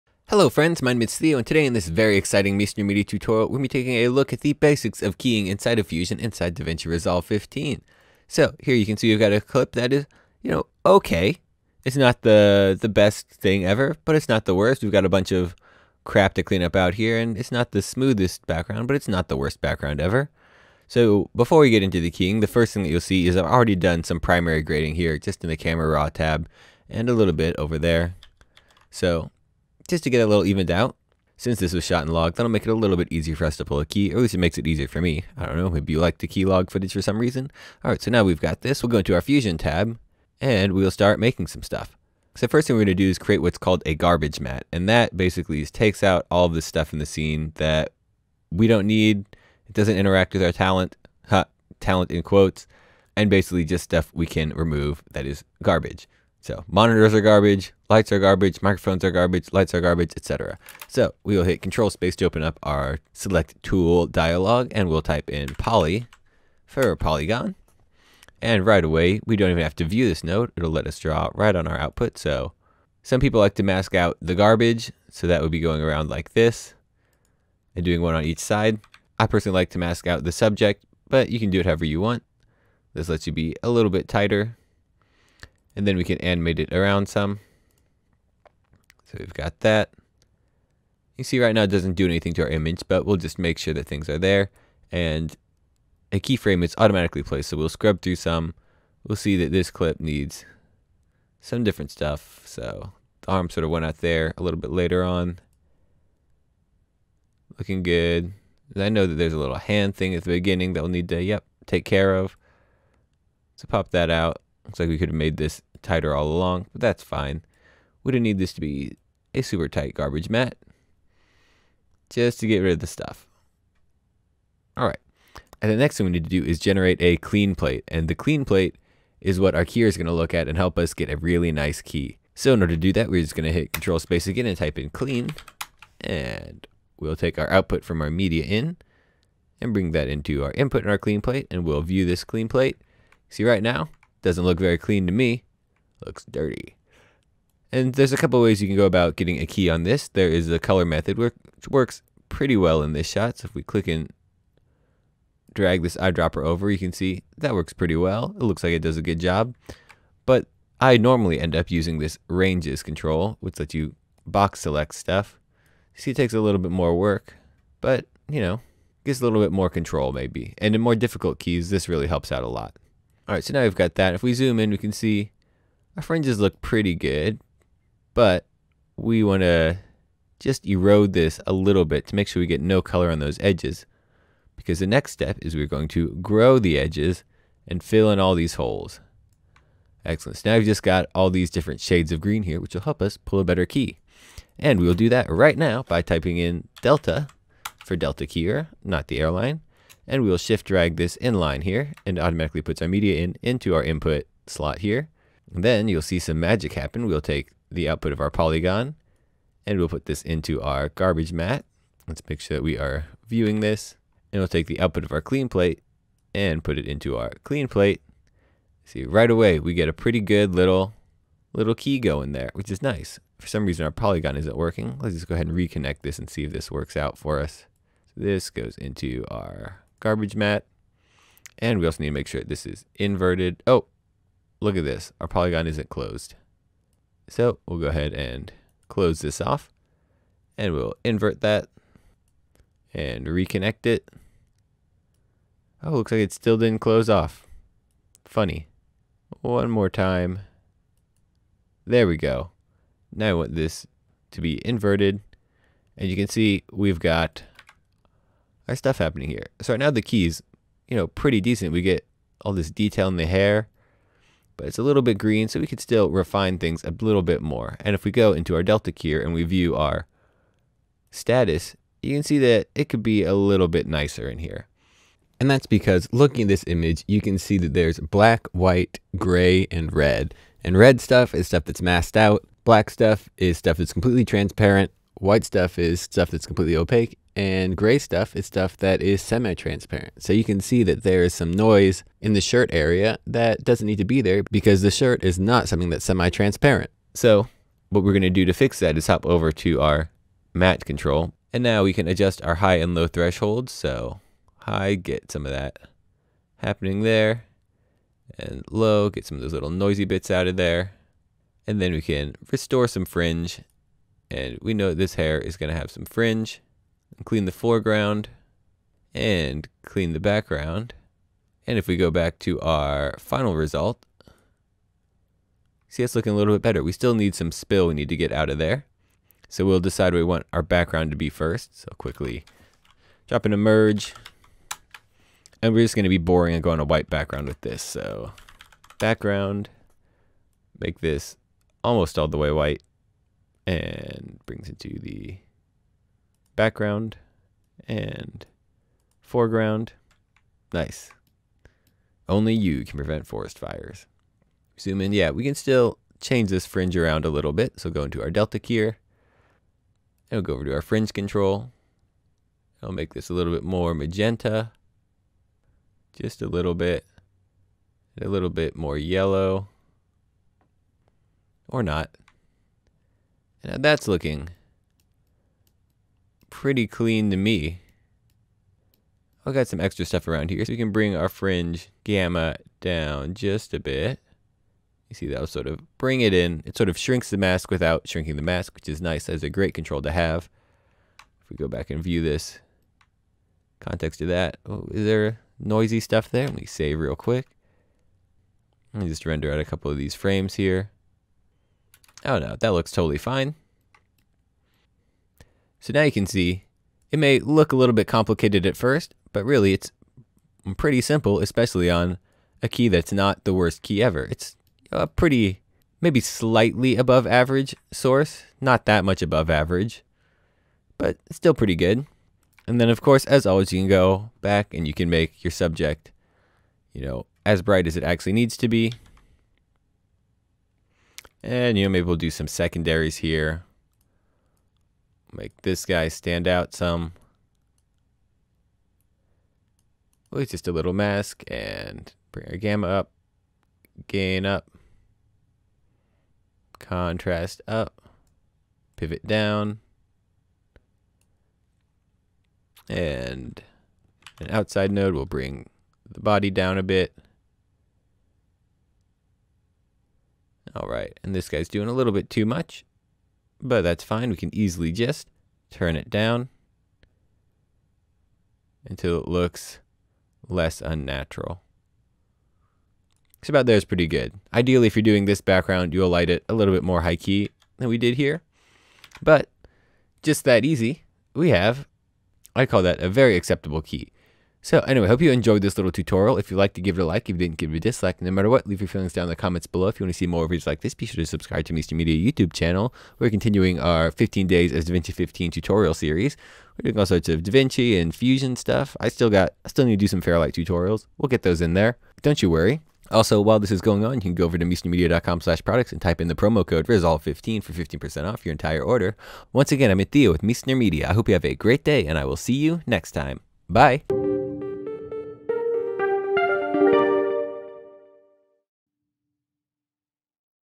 Introduction To Chroma Keying/Green Screening - Davinci Resolve15/Fusion Tutorial
In this Davinci Resolve 15/Fusion Tutorial Theo takes you through a basic chroma keying/green screen setup. The combination of Fusion's clean plate node and the delta keyer makes pulling a solid key pretty stinkin easy!